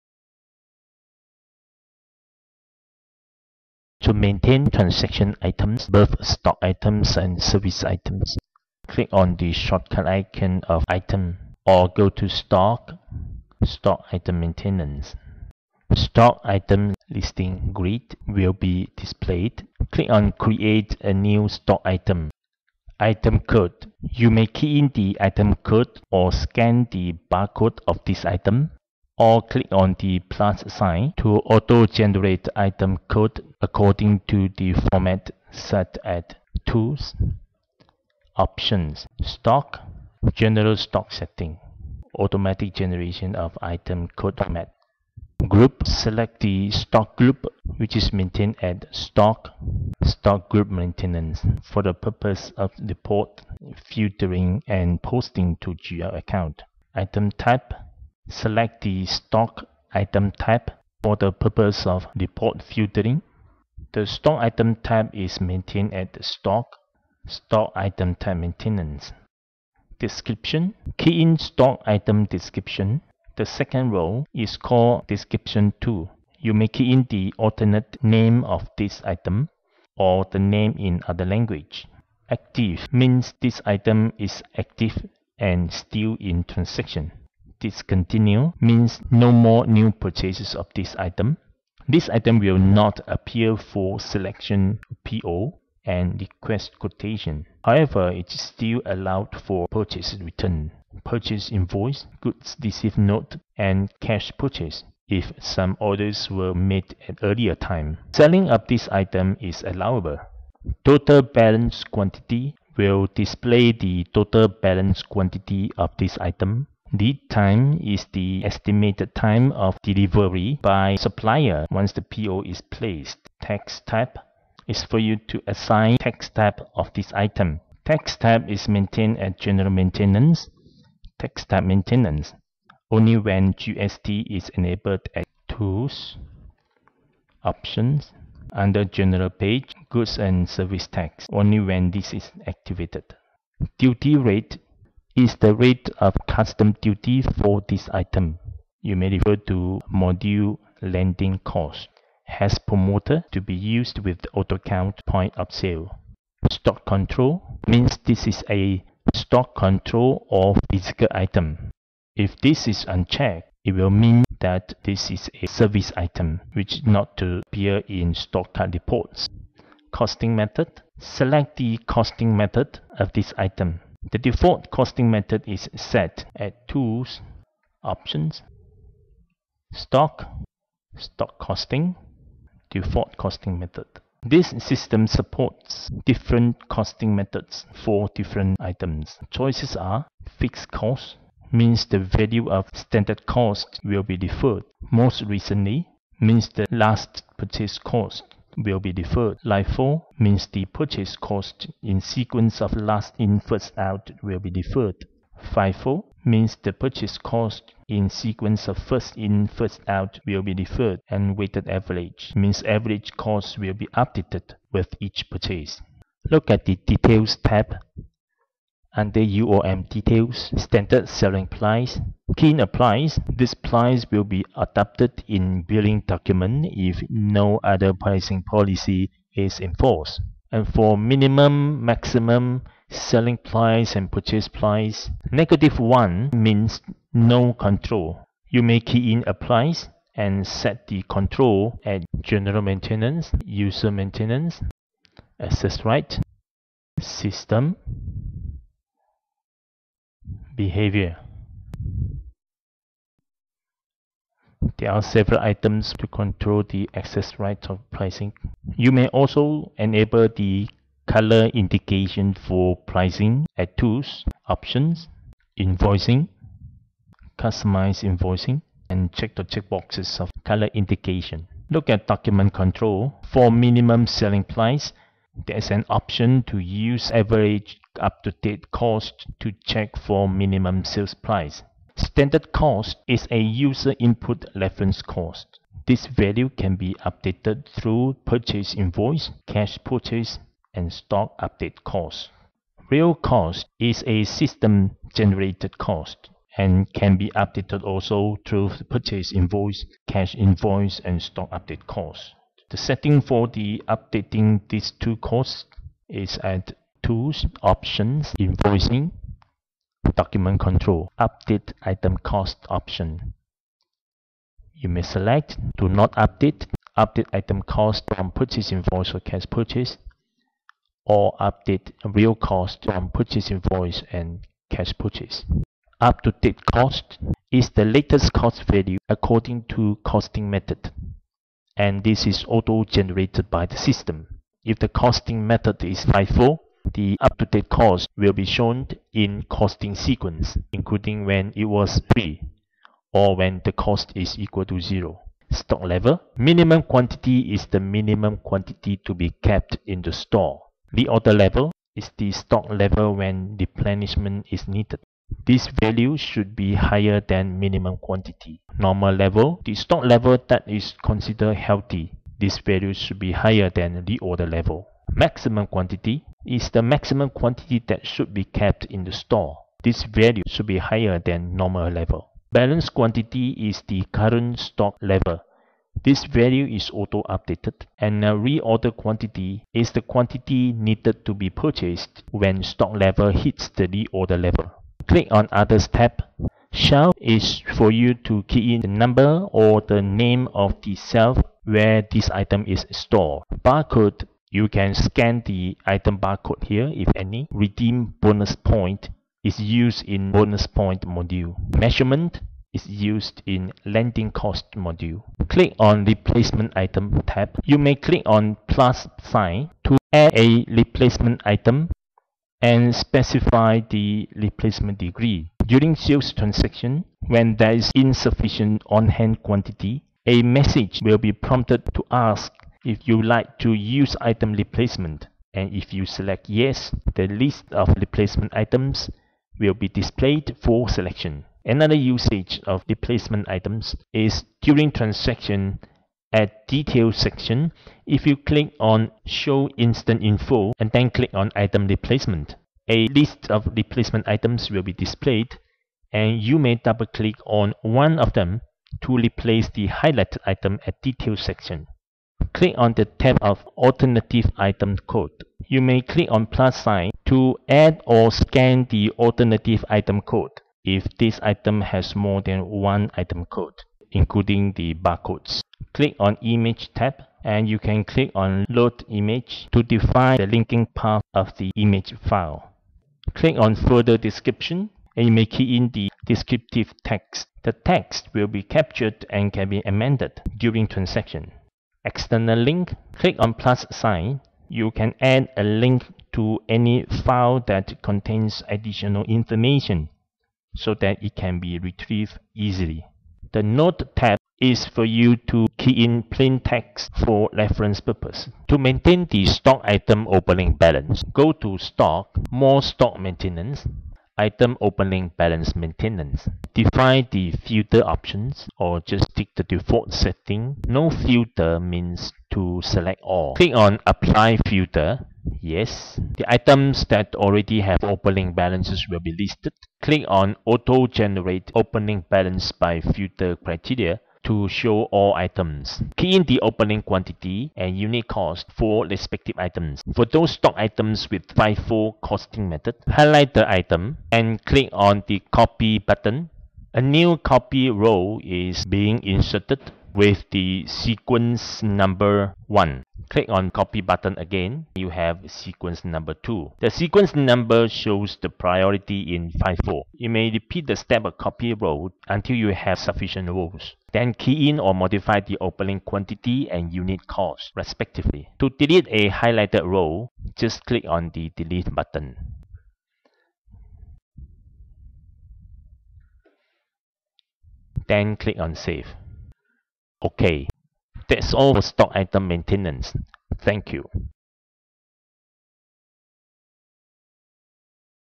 27 - AutoCount Stock Item Maintenance
• Create Stock Item
• Item code format
• Stock Group and Item Type
• Discontinue
• Costing method
• Pricing and Color indication
• Standard Cost and Up-To-Date  Cost
• Stock level
• Barcode, Replacement Item, Alternative Item  code
• Maintain Stock opening balance